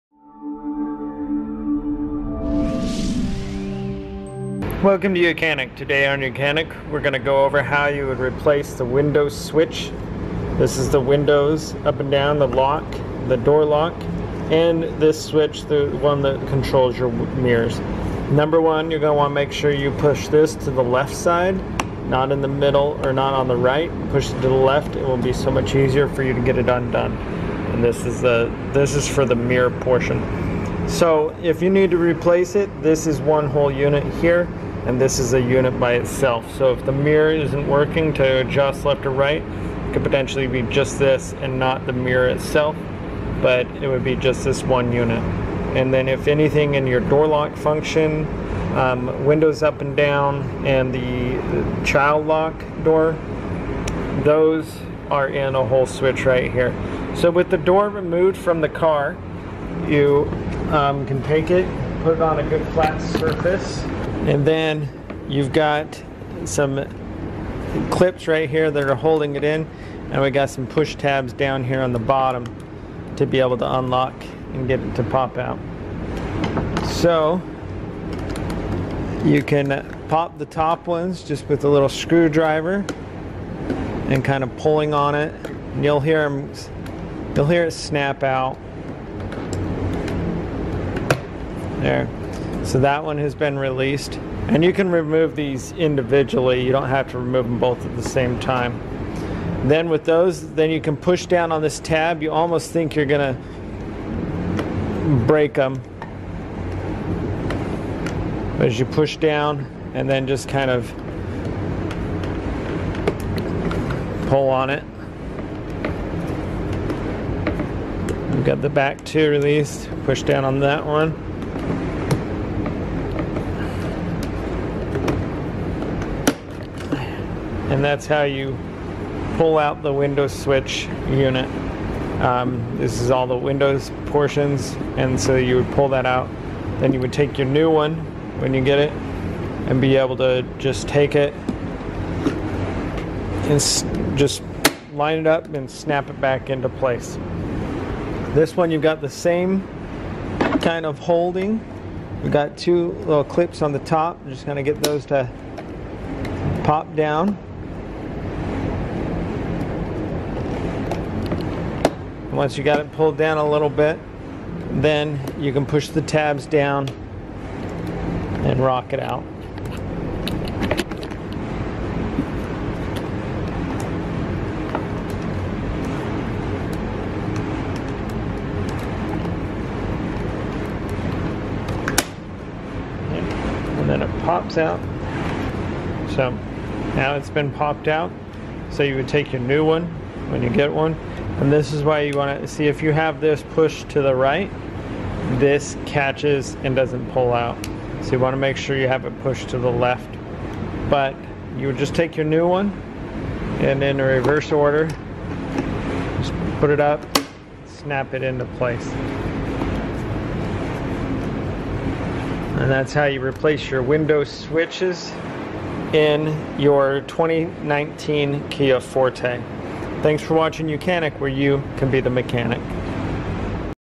2019-Present Kia Forte Window Switch Replacement
This tutorial demonstrates replacing the window switch on a 2019-present Kia Forte. The video provides step-by-step instructions and visual aids to guide the viewer through accessing the window switch assembly, removing the old switch, installing the new switch, and testing the new switch for proper functionality. The video also highlights the importance of maintaining these switches for optimal window operation and passenger comfort. The video is intended for Kia Forte owners who want to learn how to replace their vehicle's window switch and save on maintenance costs.